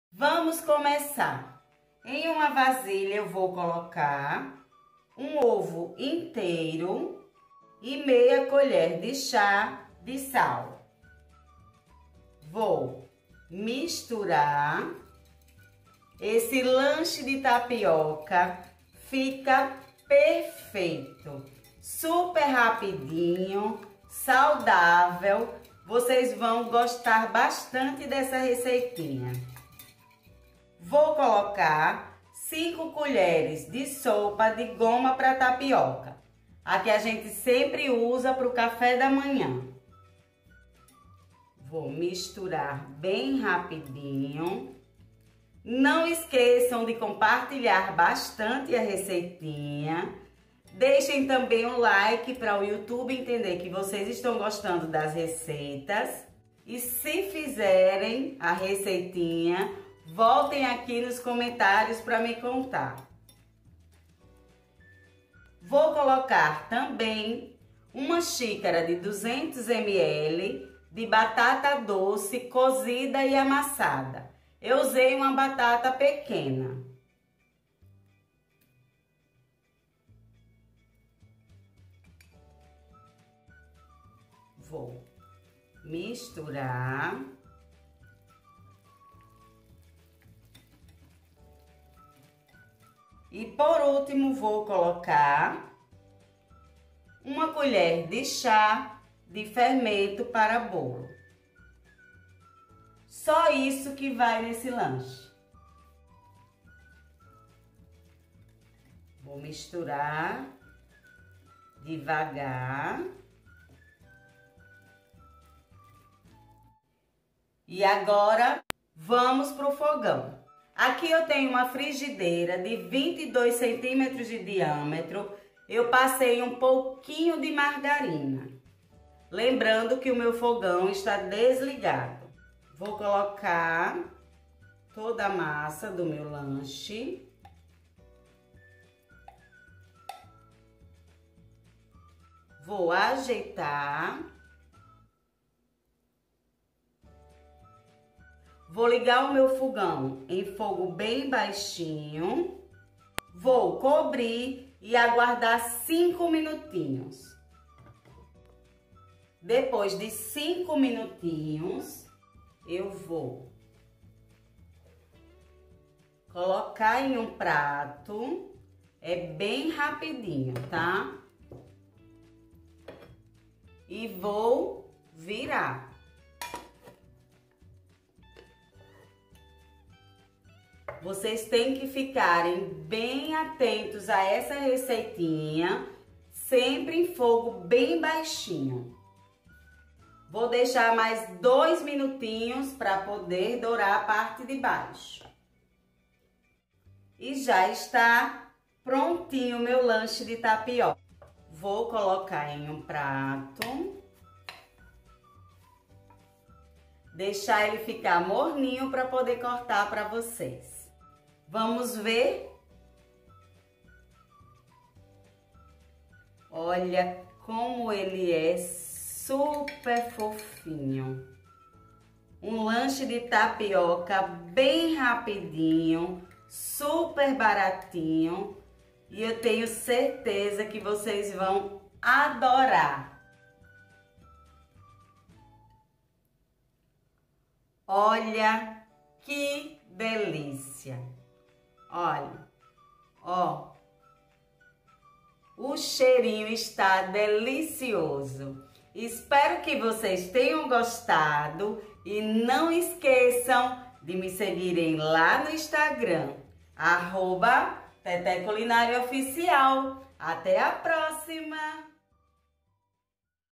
LANCHE FÁCIL DE TAPIOCA COM APENAS 1 OVO! SAUDÁVEL E DELICIOSO
OI GENTE MAIS UMA RECEITINHA COM TAPIOCA! VCS VÃO ADORAR O SABOR FICA INCRÍVEL E É SUPER E ECONÔMICA! COMPARTILHEM MUITOOOOO 🧡

🧡INGREDIENTES
1 OVO
5 COLHERES DE SOPA DE GOMA HIDRATADA PRA TAPIOCA AQUELA QUE USAMOS NO CAFÉ DA MANHÃ
MEIA COLHER DE CHÁ DE SAL
1 XÍCARA DE 200ML DE BATATA DOCE COZIDA E AMASSADA
1 COLHER DE CHÁ DE FERMENTO PARA BOLO